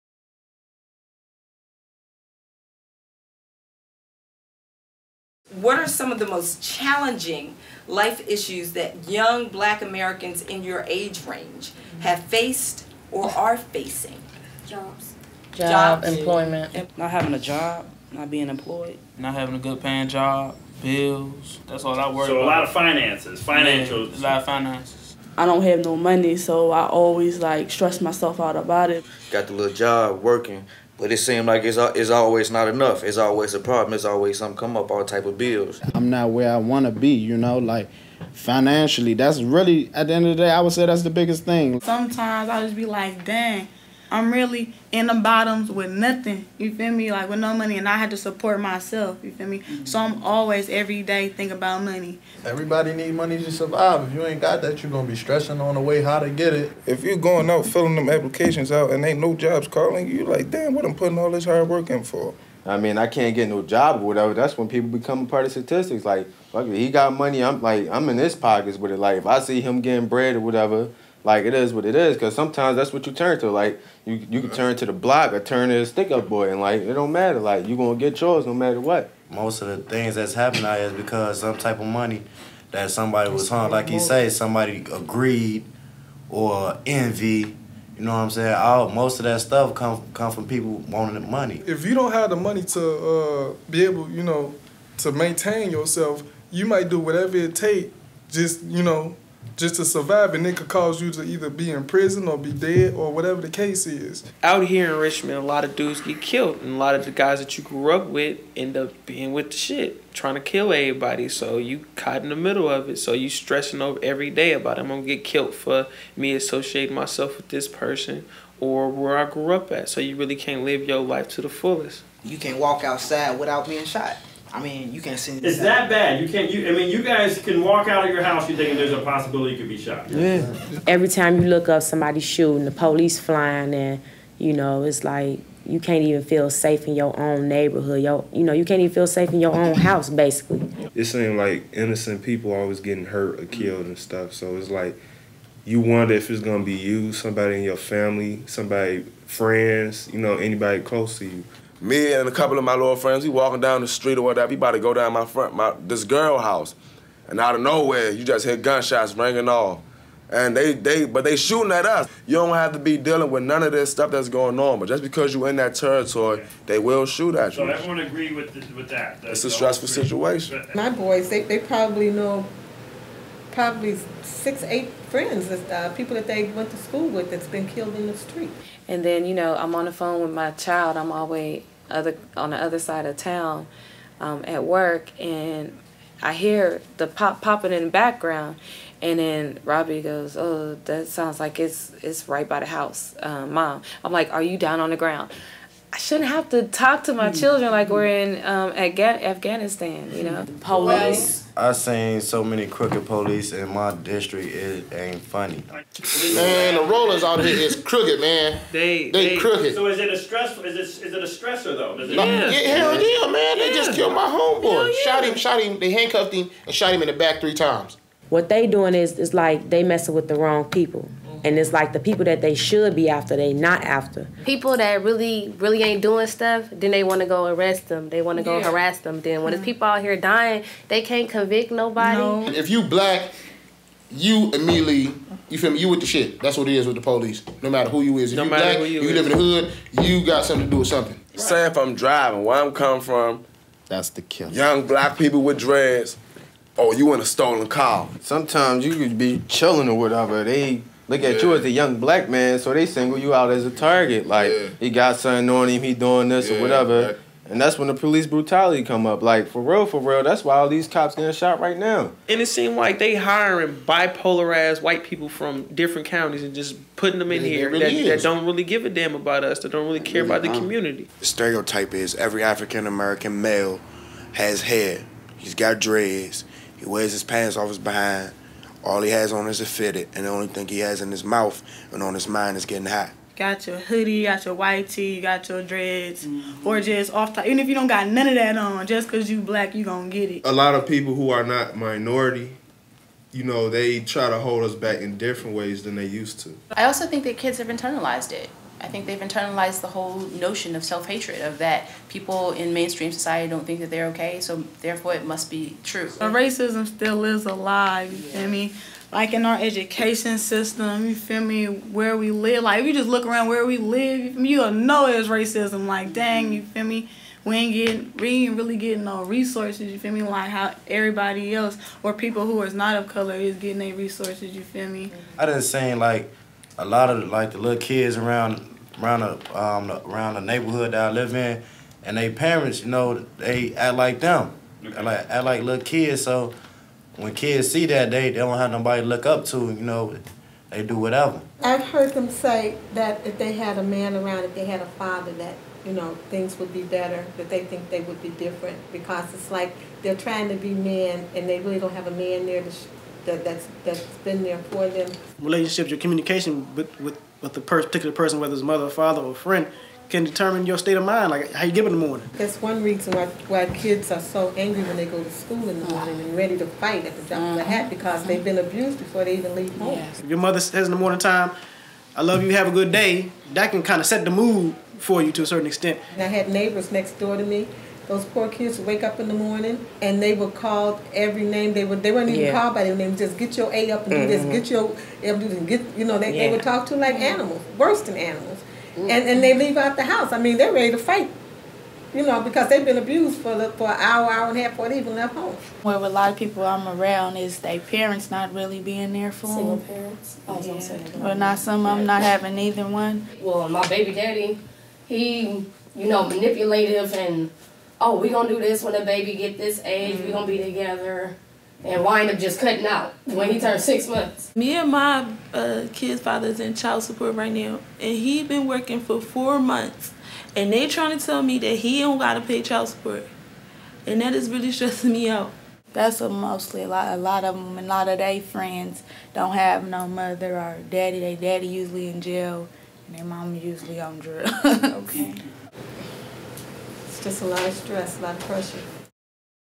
Moving Beyond Survival Mode - Stressors
"Moving Beyond Survival Mode," explores how living in low-income, underserved communities puts individuals at risk for stress, ongoing trauma, depression and even mental health problems. This video showcases the voices of Americans who too often fall through the cracks. Young Black Americans -- who feel beaten down by the struggle to survive poverty, broken homes, child abuse, violent communities, poor schools, institutional racism, police harassment and other social ills -- share their stories and their emotional "injuries."

These service providers and young women and men who live in Philadelphia, Washington DC, Chicago and the Oakland area tell the story of what it's like living in survival mode, when their neighborhoods are overrun by violence, poverty and drugs.